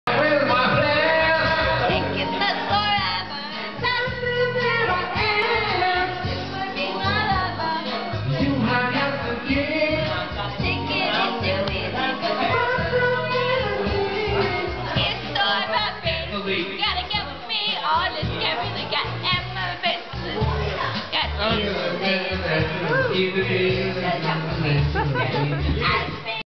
Karaoke at "Lover Doll"
Sarah and some other karaoking types sing the Spice Girls.